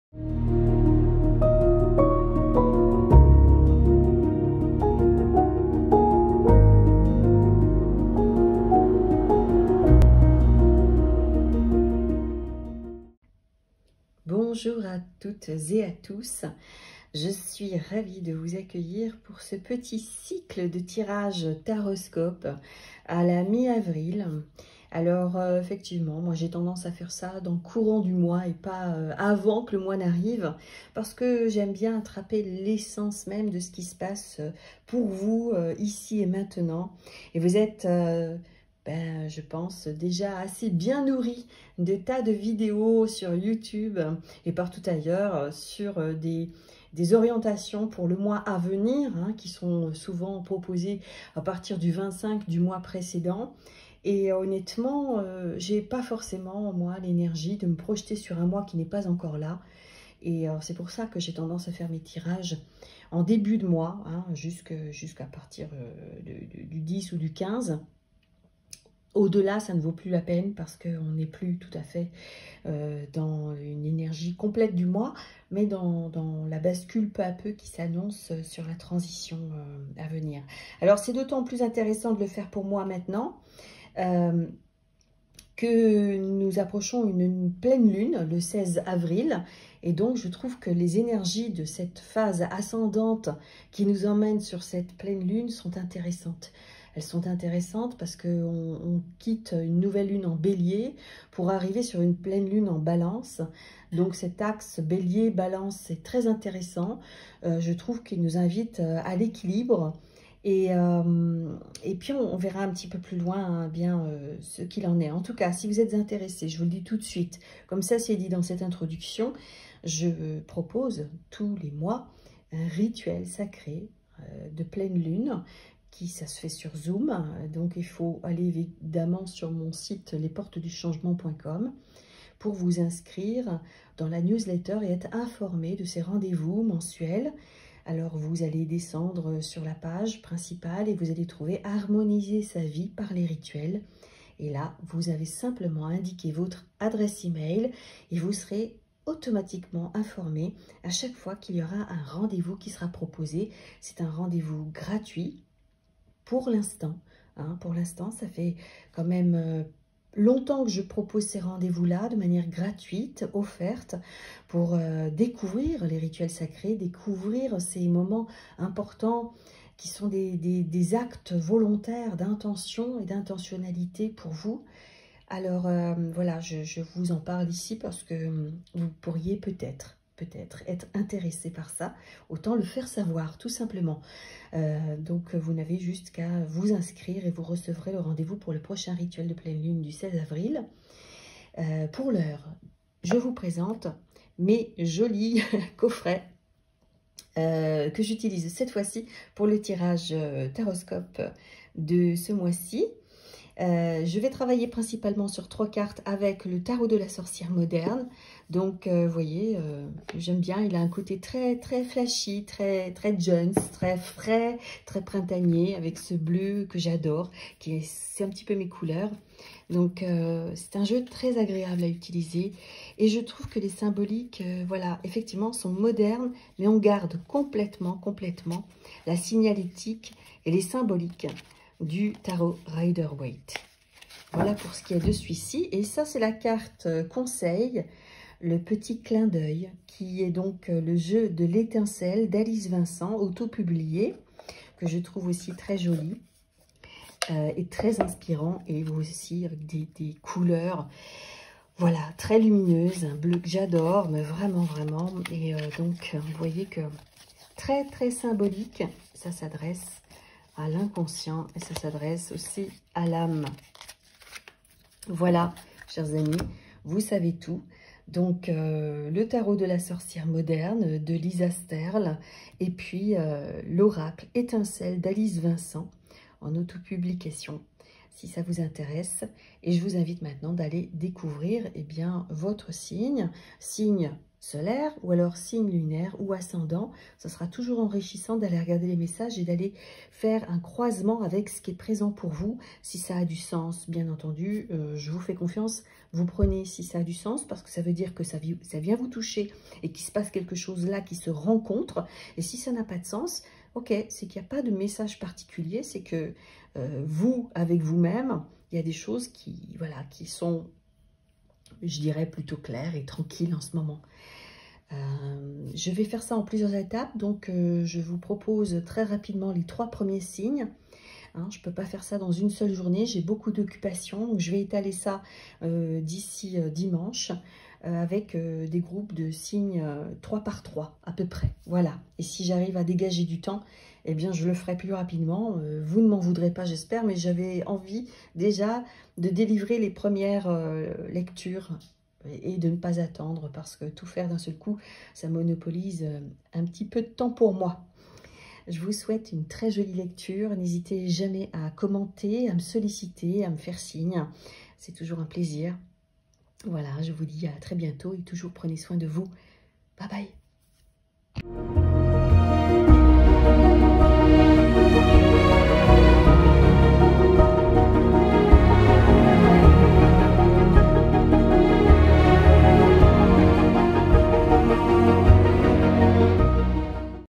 Introduction 🧚 Taroscope Avril 2022
Cette 1ère vidéo est l'introduction de mes lectures symboliques d'avril 2022 (mi-avril / mi-mai).

Je vous présente le tarit et l'oracle que j'ai choisis d'utiliser cette fois ci.

Je vous fais part également du prochain rituel gratuit de pleine lune que j'organise samedi 16 avril prochain à 20H en direct sur Zoom.

Chaleureusement ♡
Béatrice